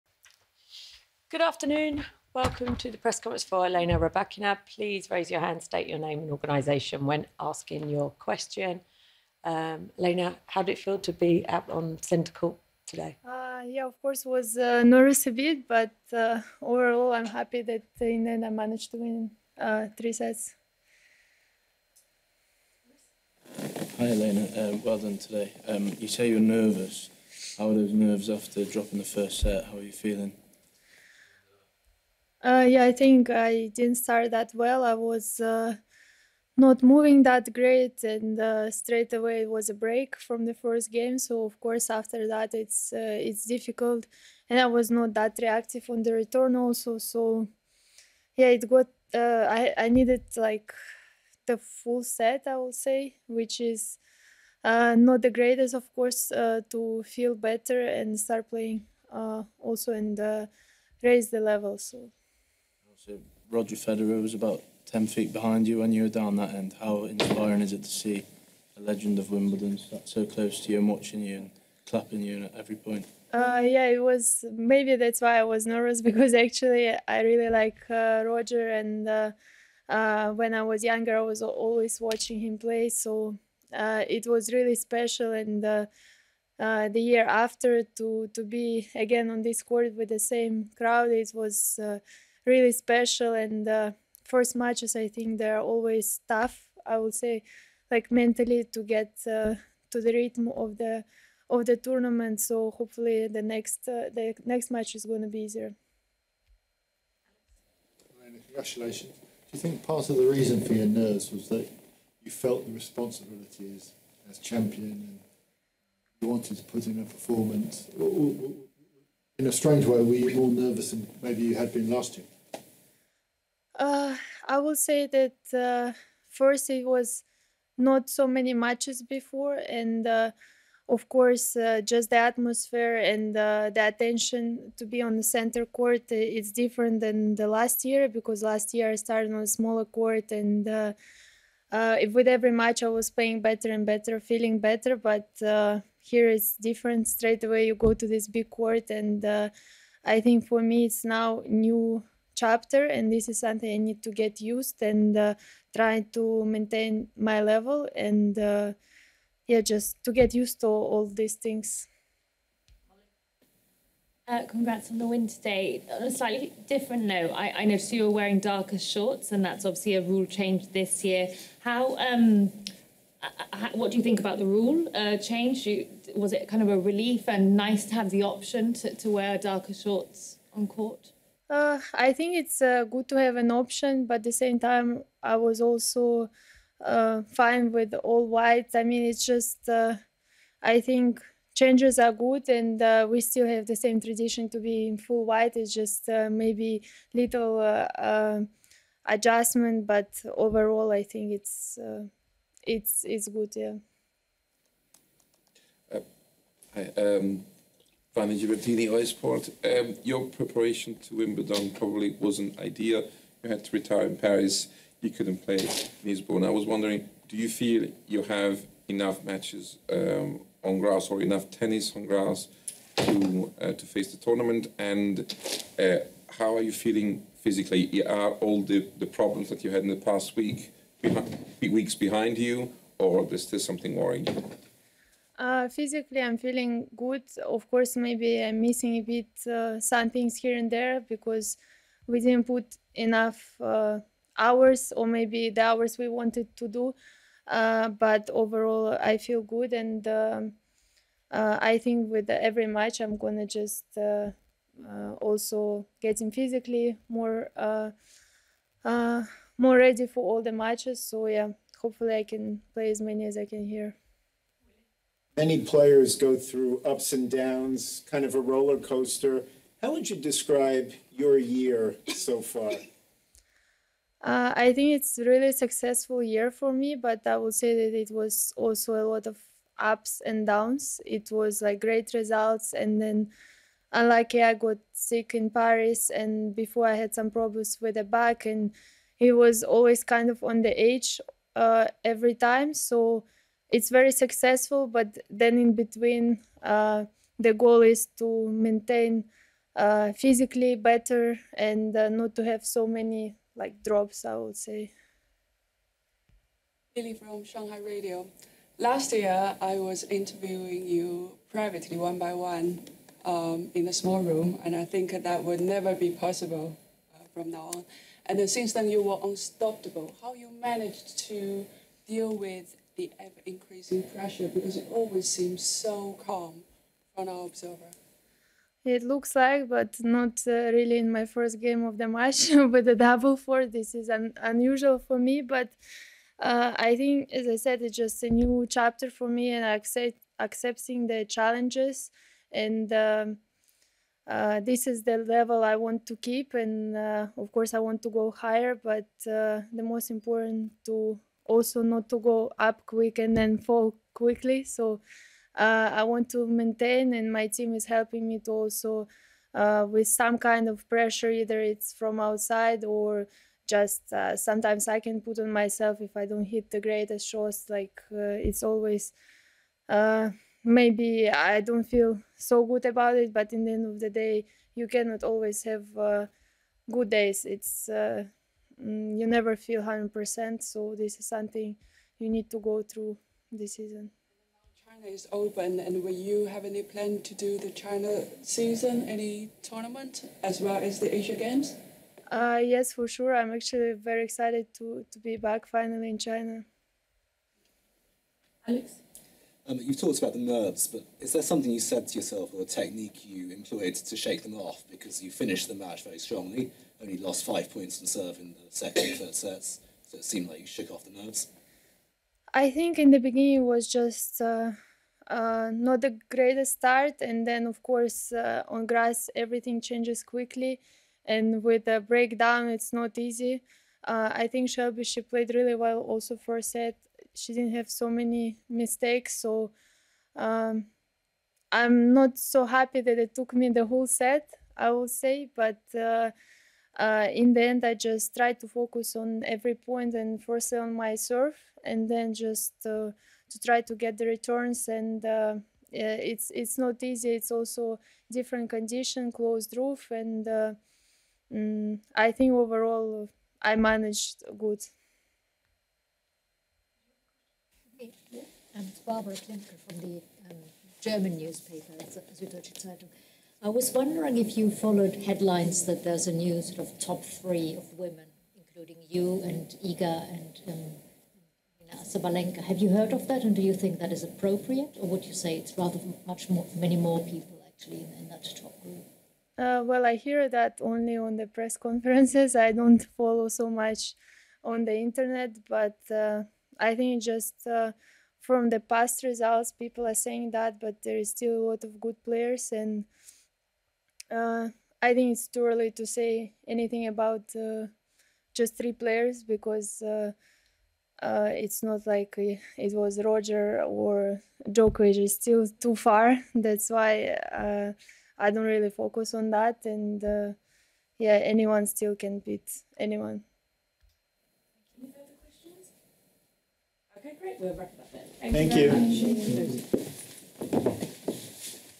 Elena Rybakina | First Round Press Conference | Wimbledon 2023
"The atmosphere and attention on Centre Court is different this year."

Reigning Ladies Champion Elena Rybakina reflected on a tough First Round match and compares her experience from last year's Championships to the present day.